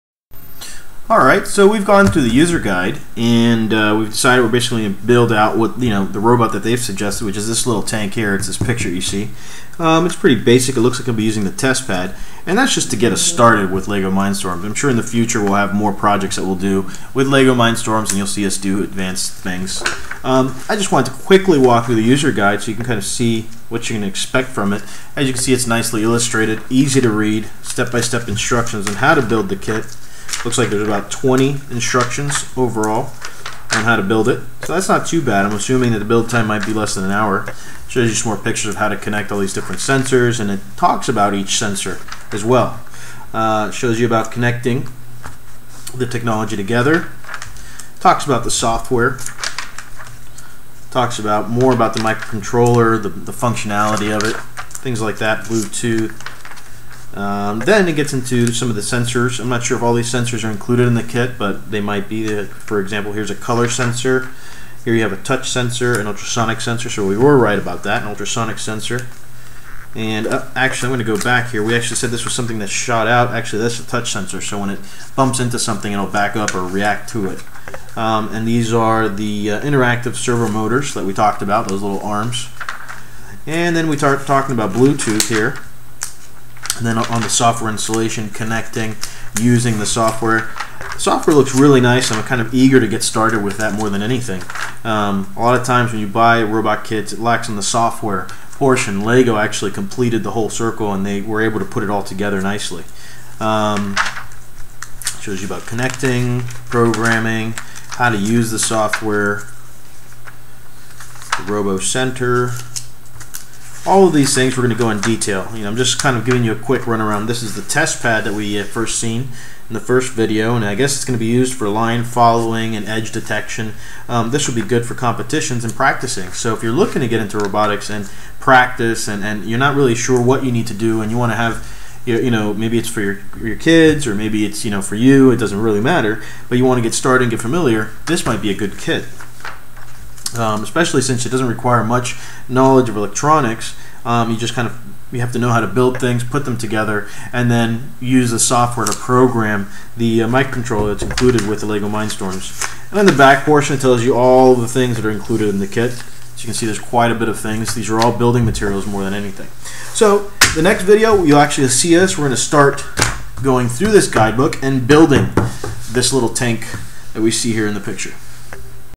Lego Mindstorms Review - Part 3 - RUG Community Robot Review
This is a multi-part review of the Lego Mindstorms Robot Kit